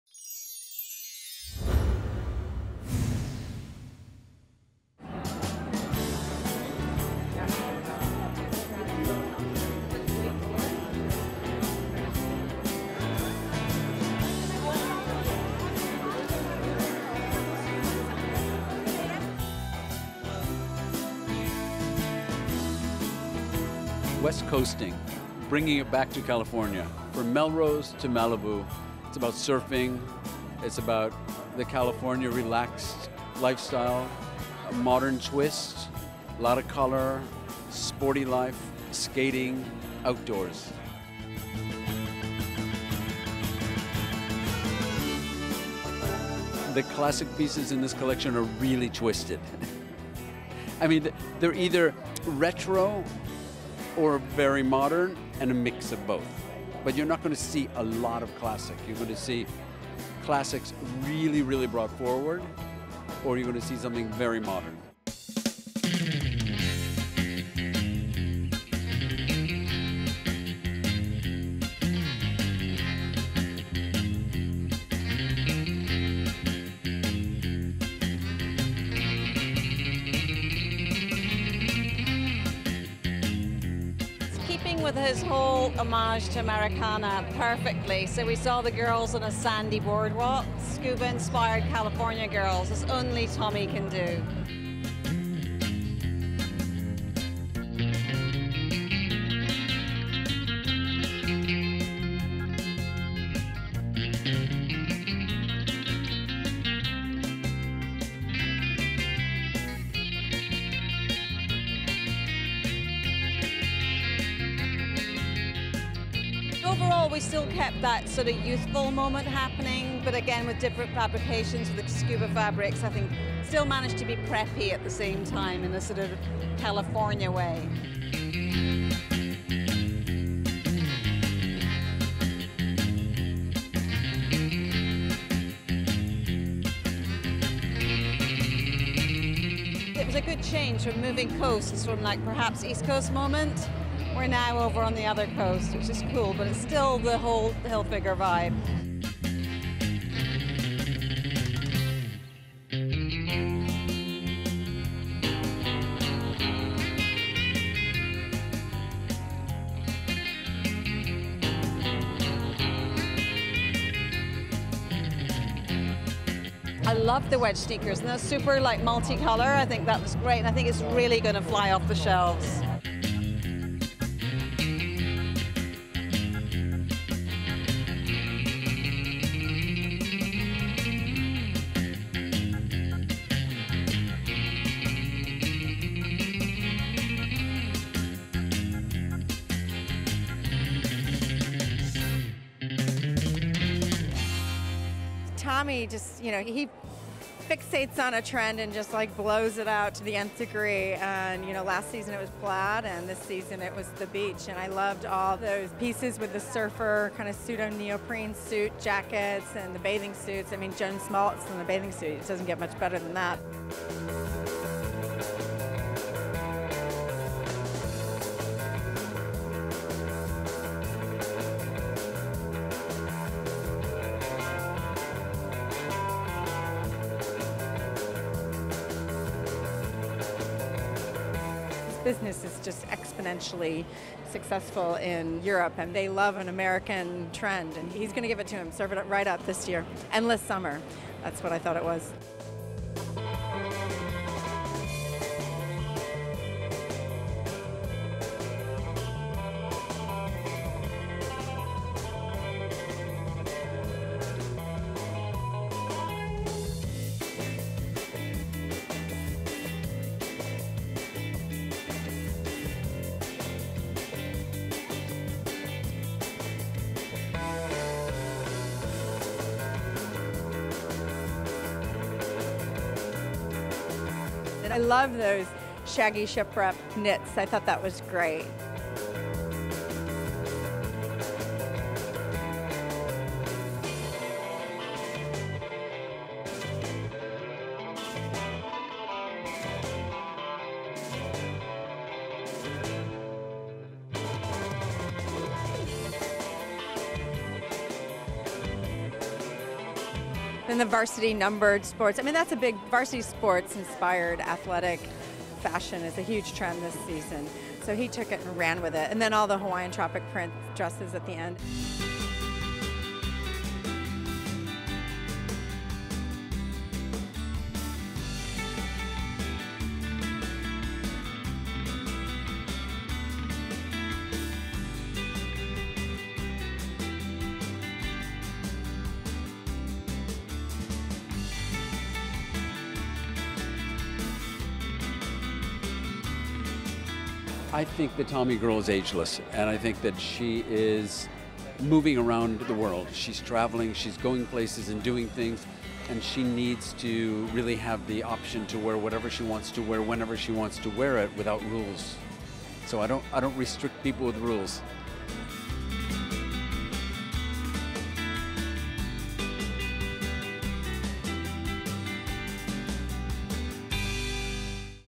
Tommy Hilfiger Spring/Summer 2014 - Videofashion
For Spring 2014, Tommy Hilfiger goes west coasting along the sandy beaches of California. Surf, scuba and skating cultures come to life on the runway with boldly-hued neoprene jackets and sports jerseys topped with youthful wedge sneakers.